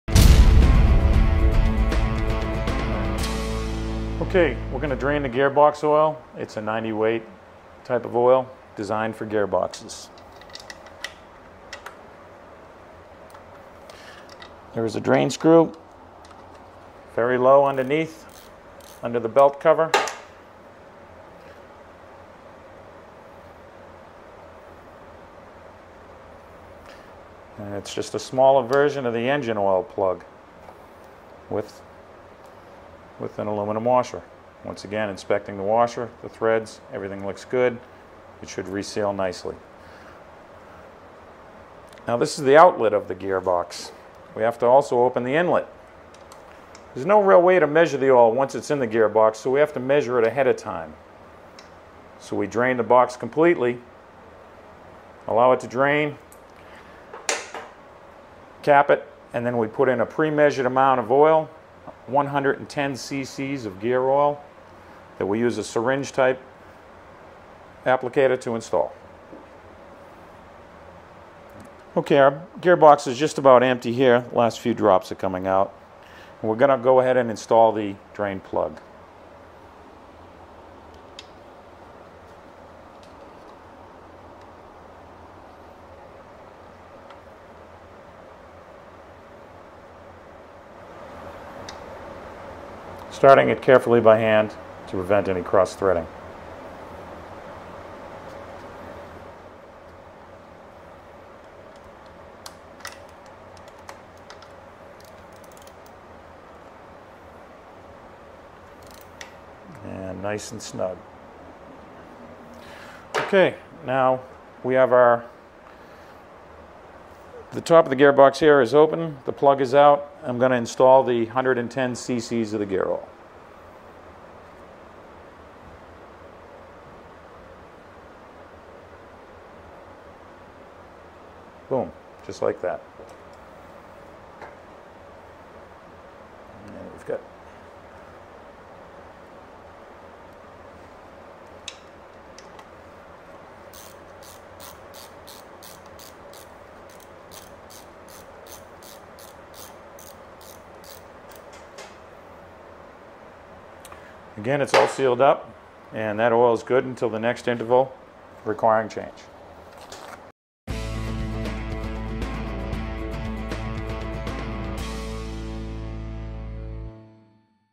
Basic Scooter Maintenance - Changing Gear Oil
Learn how to easily change the gear oil on your scooter